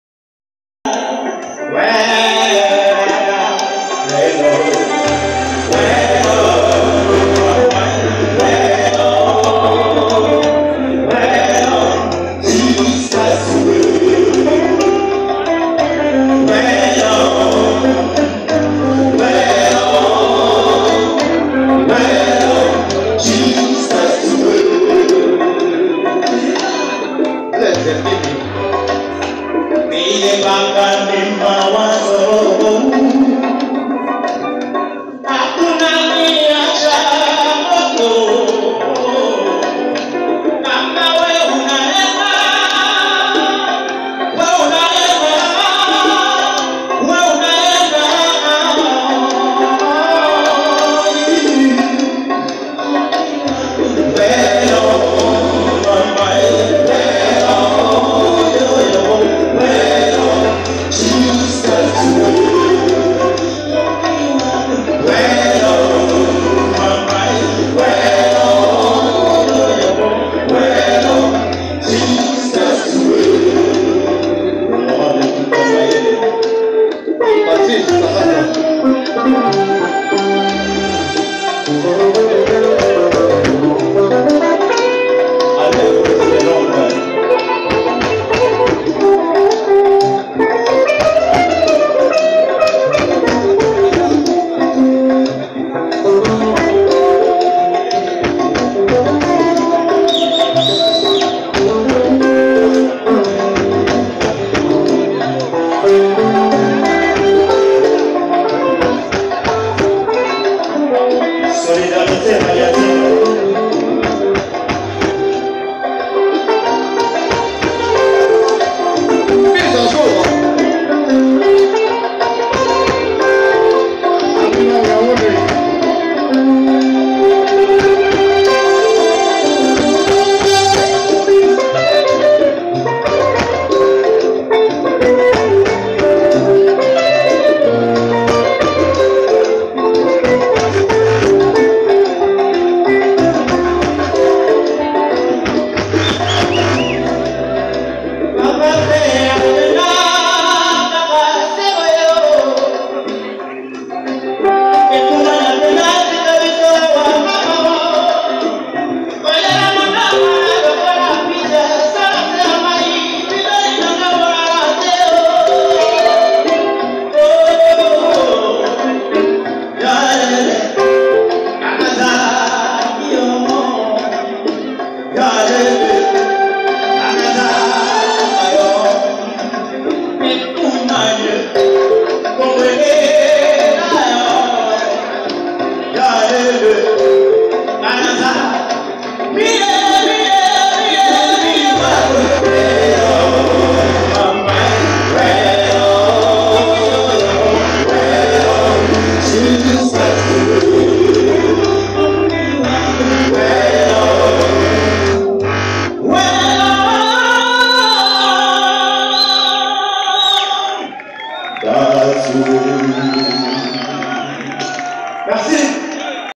premier concert de Joe le noir mise à jour alelisi batu n'a Gare centrale
le retour de Joe le noir mis à jour  festival rumba n'a rumba à la place de la gare centrale  .alelisi batu